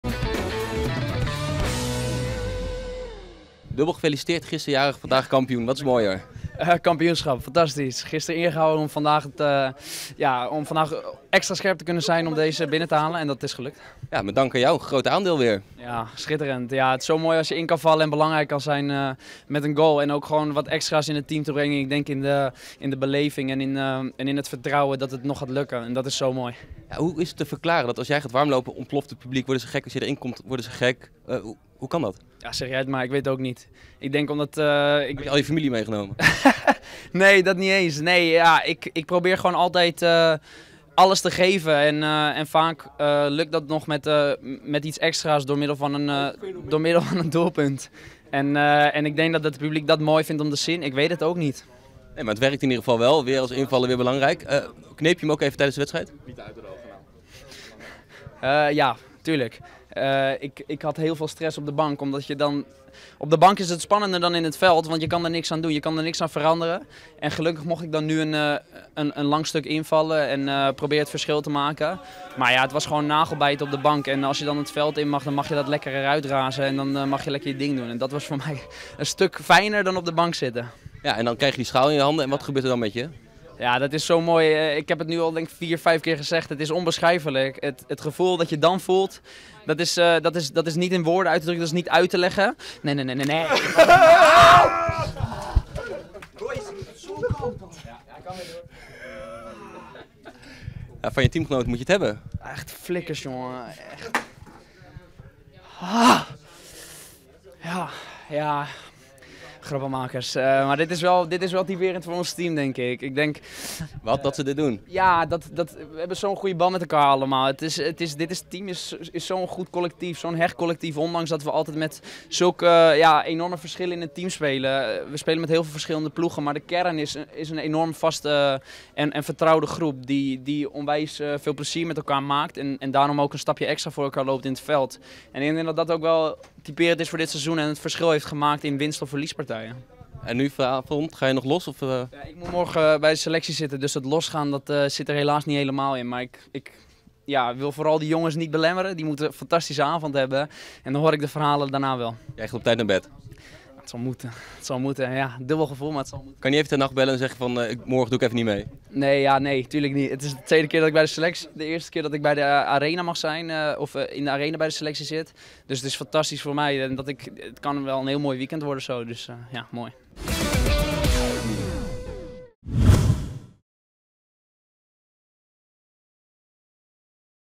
Sierhuis na bierdouche: ''Dit is typerend voor ons team’’
Clubinsider Daniel Schoonenboom sprak met Kaj Sierhuis na afloop van het behalen van het kampioenschap van Jong Ajax.